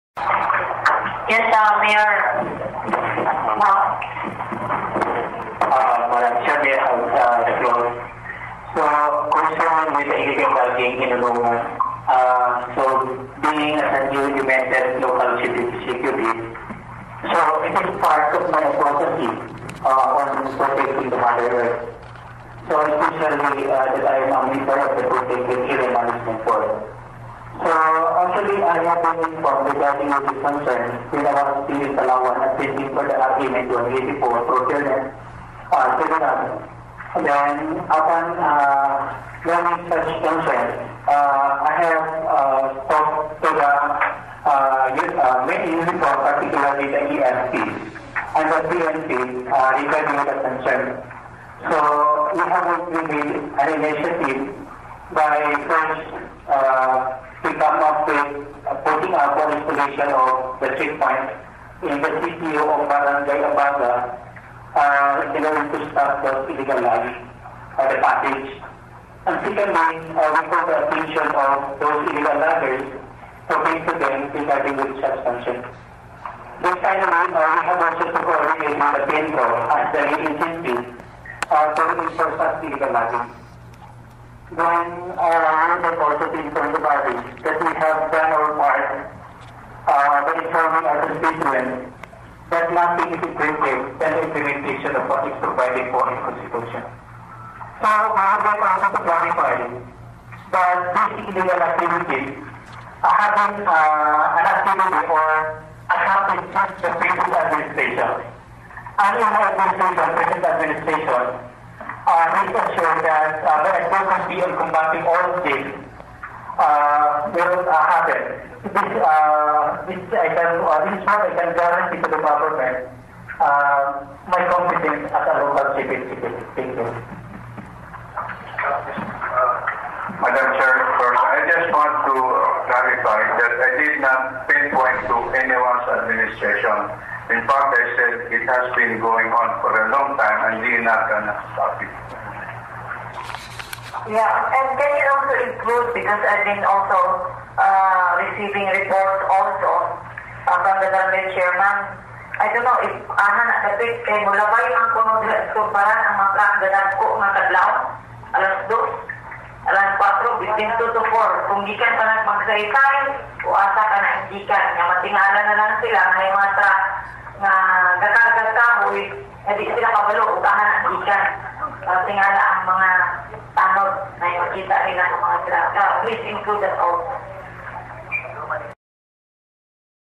Response to Congressman Abdullah Dimaporo Re Illegal logging
Response of Hon. Mayor Marcos Mamay of Nunungan, Lanao del Norte regarding Illegal Logging